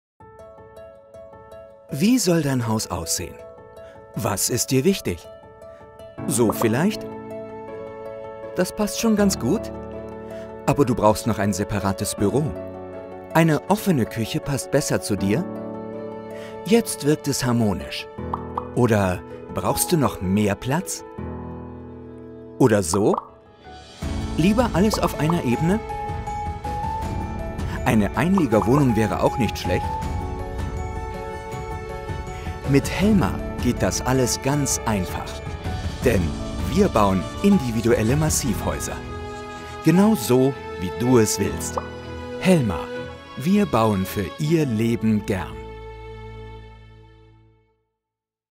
Individualität beim Hausbau mit HELMA - LIVE SCRIBBLE PREMIUM Erklärfilm mit 3D-DETAILS - EXPLANIDEO
Für unseren innovativen Kunden HELMA dürfen wir insgesamt 3 Videos produzieren. Das erste Video ist fertig und wir erklären darin die Individualität, die HELMA beim Hausbau bietet!

Einige "3D"-Elemente machen ein ganz besonderes Video daraus!

Stil: LIVE SCRIBBLE PREMIUM + 3D-SPEZIAL

Sprache: Deutsch

Film 1 von 3

Produziert von: EXPLANIDEO